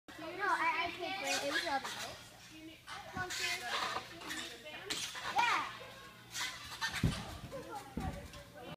Kyrsten Leazer new bar combination
Level 10 CEG gymnast, Kyrsten, putting her new bar combo together!